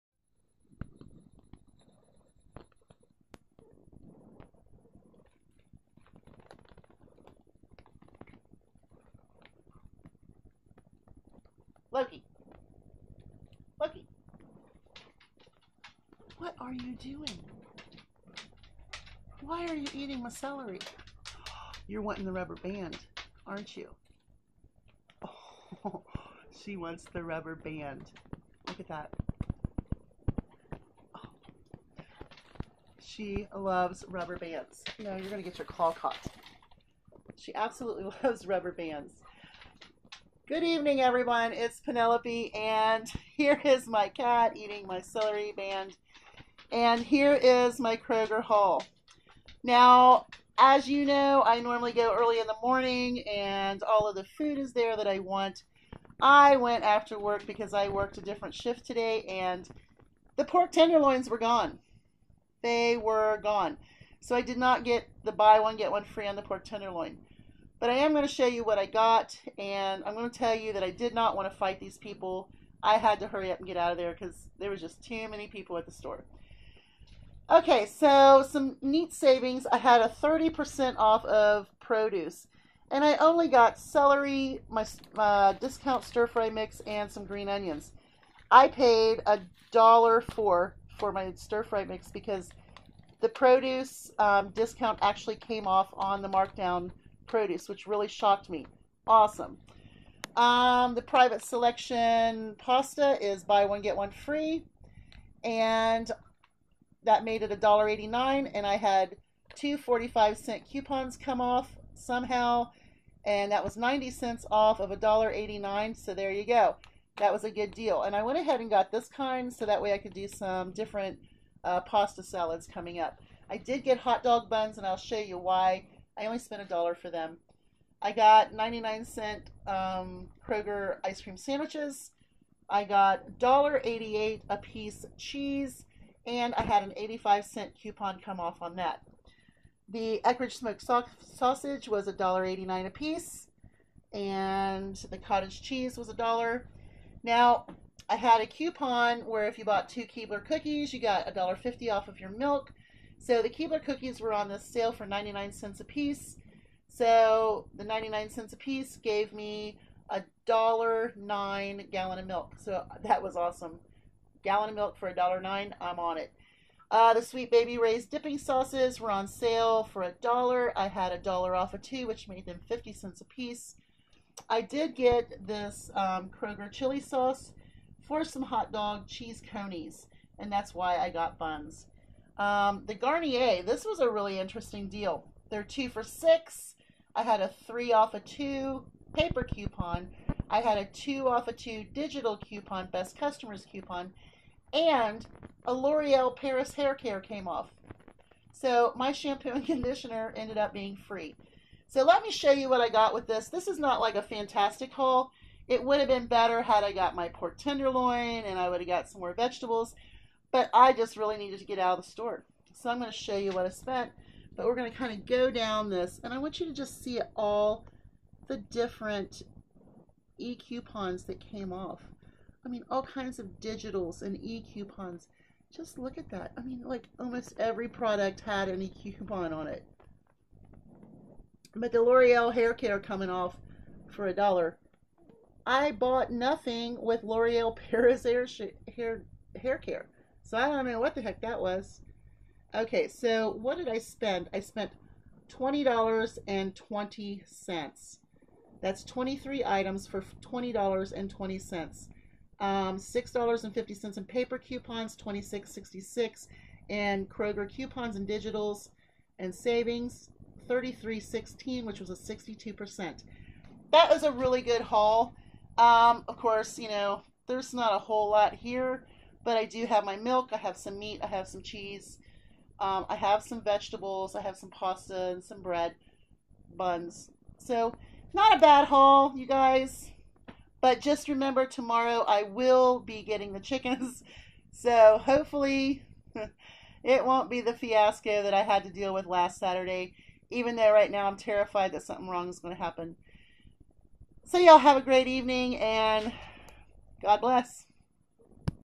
Kroger Haul & Loki Antics
Cat tries to steal rubber band during haul video